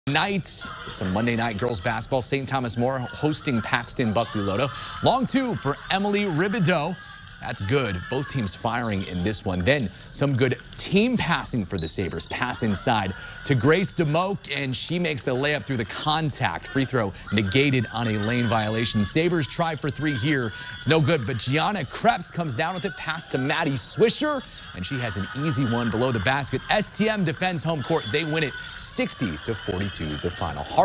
HS scoreboard (1-30-23)
Scores from around Central Illinois, including highlights of St. Thomas More's win over Paxton-Buckley-Loda.